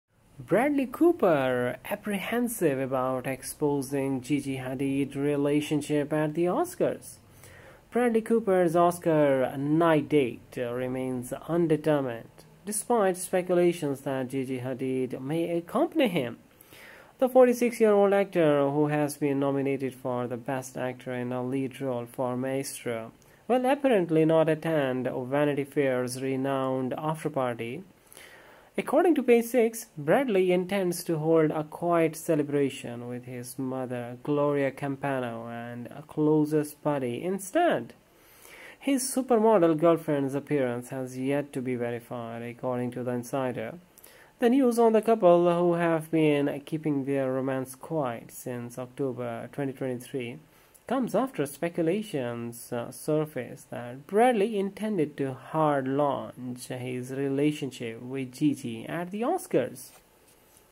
Bradley Cooper apprehensive about exposing Gigi Hadid relationship at the Oscars?
Bradley Cooper's Oscar night date remains undetermined, despite speculations that Gigi Hadid may accompany him.
The 46-year-old actor, who has been nominated for Best Actor in a Lead Role for Maestro, will apparently not attend Vanity Fair's renowned afterparty.
According to Page Six, Bradley intends to hold "a quiet celebration" with his mother Gloria Campano and closest buddy instead.
His supermodel girlfriend's appearance has yet to be verified, according to the insider.
The news on the couple, who have been keeping their romance quiet since October 2023, comes after speculations surfaced that Bradley intended to "hard launch" his relationship with Gigi at the Oscars.